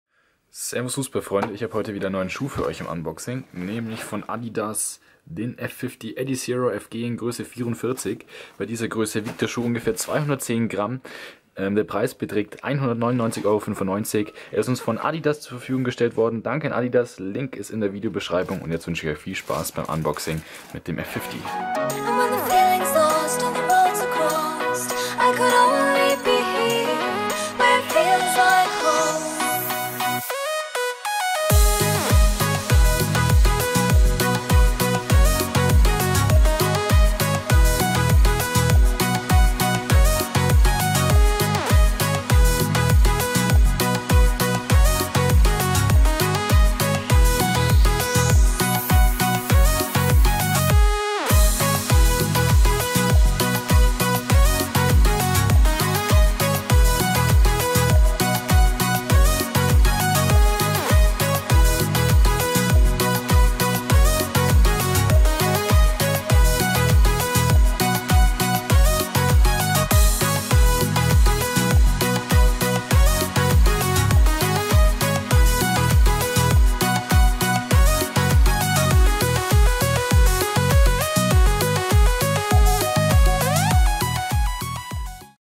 Unboxing: Adidas F50 Adizero FG
Heute haben wir den neuen Adidas F50 Adizero für euch im Unboxing! Viele Bundesligaprofis tragen diesen Fußballschuh! Gefällt er euch auch?

Logo erstellt von Kirill Singer! Vielen Dank für die Unterstützung!